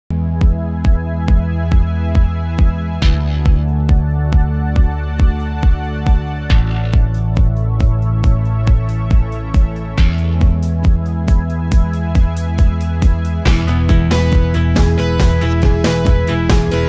Sapatilha Slipper Moleca Boca 6060430902
Sapatilha Slipper Moleca 5255.215, feita em material sintético com bico arredondado. O forro é em material têxtil e a palmilha é macia em PU. Solado é em borracha. Modelo com estampa de boca.

Medidas Aproximadas
Altura do salto: 1,5cm | Largura da sola: 8,5cm | Peso: 322g.